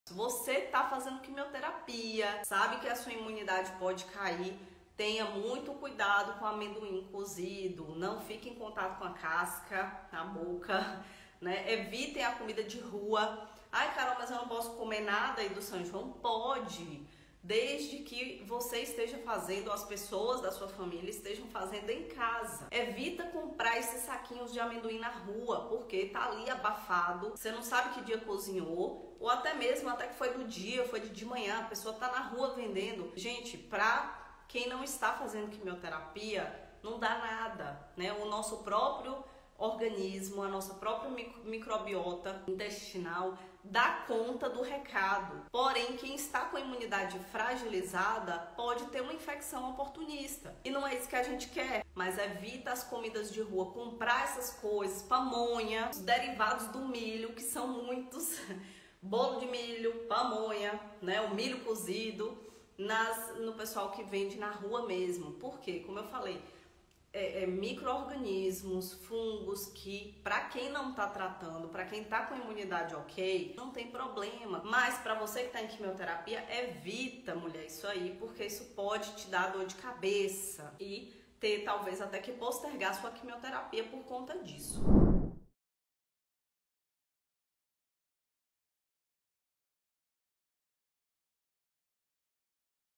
Está em quimio para o câncer de mama? Esse vídeo é pra você
Você pode curtir as festas juninas, porém com alguns cuidados com o que você consome e onde consome. Evita comidas de rua, onde você não sabe como foi preparado. A quimio pode deixar sua imunidade mais baixa e você fica mais susceptível a infecções.